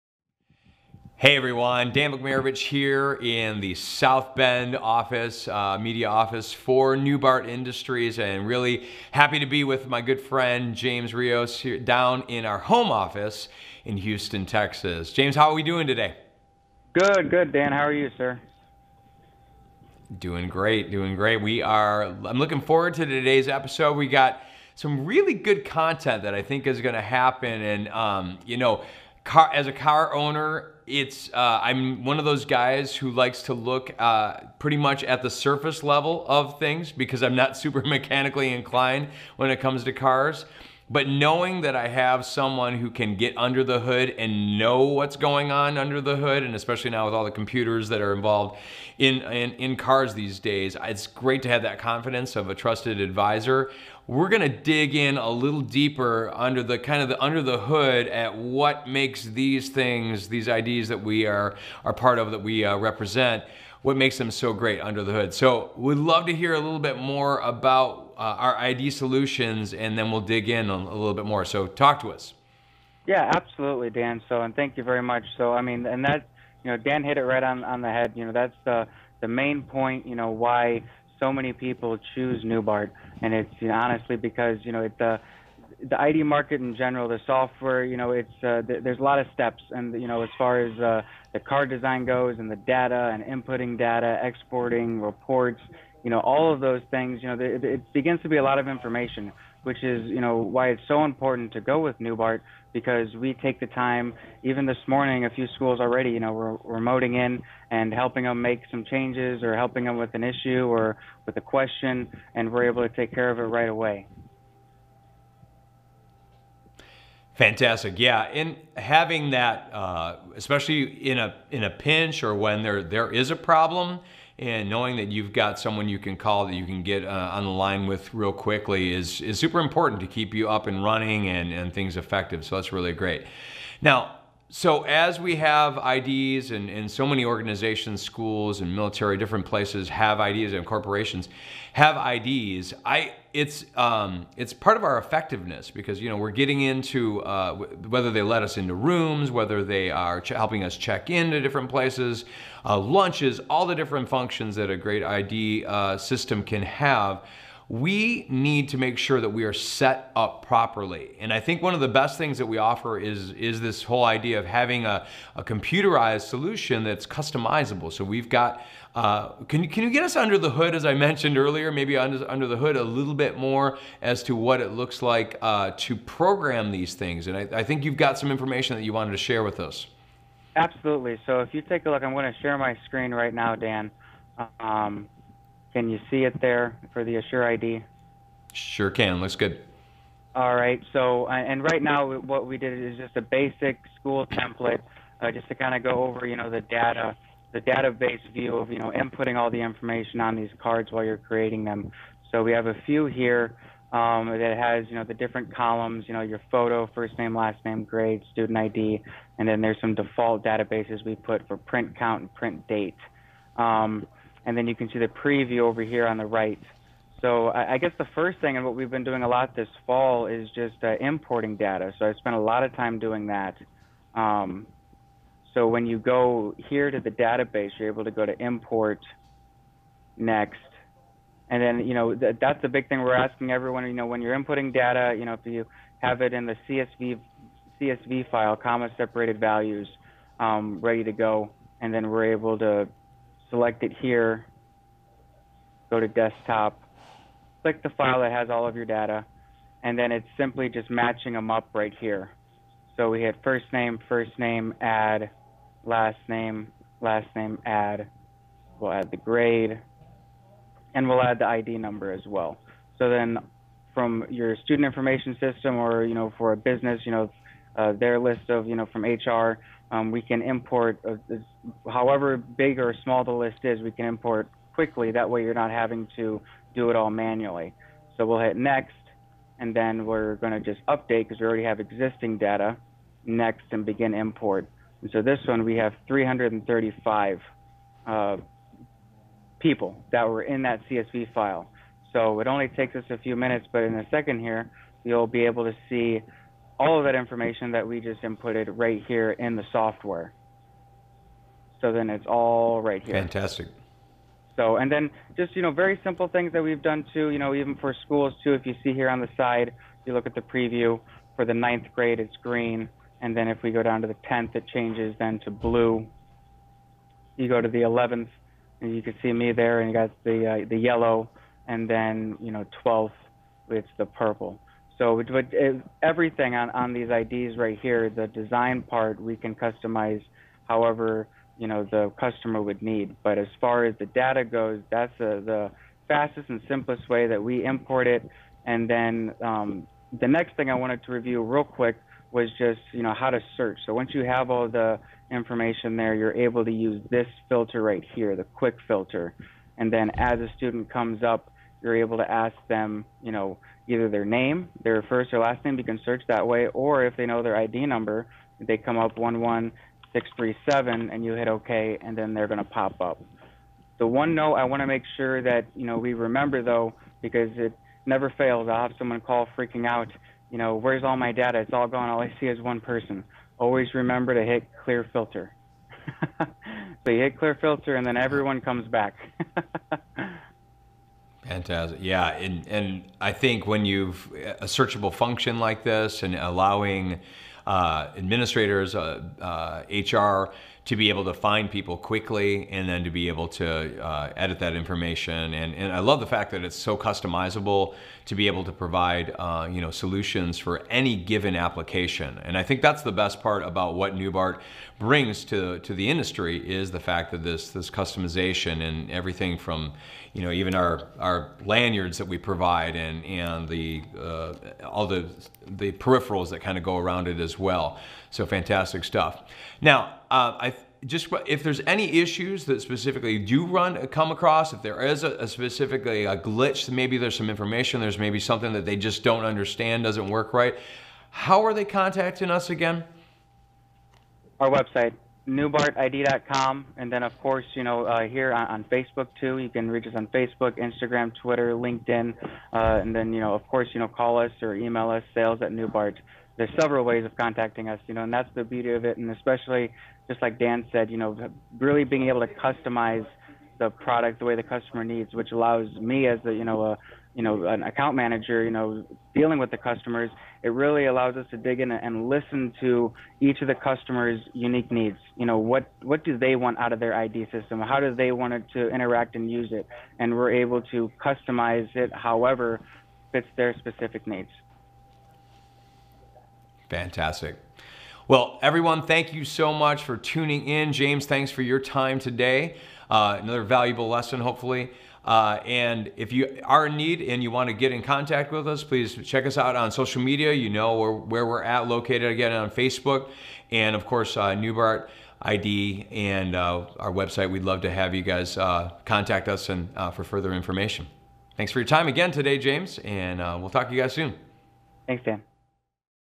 Newbart: Asure ID Solutions!
We talk about these little plastics cards a lot, but how do you create one personalized to fit the needs of your organization? Today, we'll be discussing setting up your ID solutions through Asure ID!